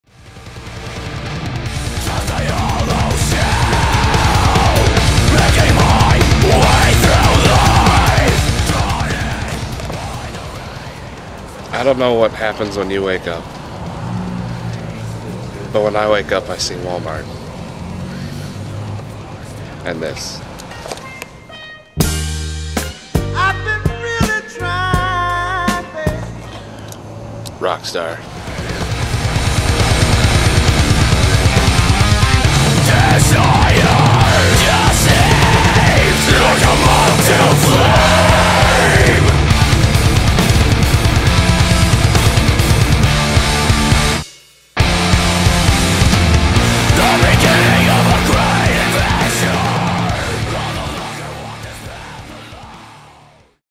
Iron Thrones - How To Survive On Tour (Part 1 - "Sleeping")
"When you're unable to afford a hotel, and can't convince anyone at the show to let your stinky, bum-looking band sleep on their floor, look for the nearest Wal-Mart.  Unlike most businesses, the majority of Wal-Mart stores allow RV'ers (and bands) to park & sleep in their parking lots overnight.  Plus, lots of
them have Subways now, who serve cheap breakfast and coffee."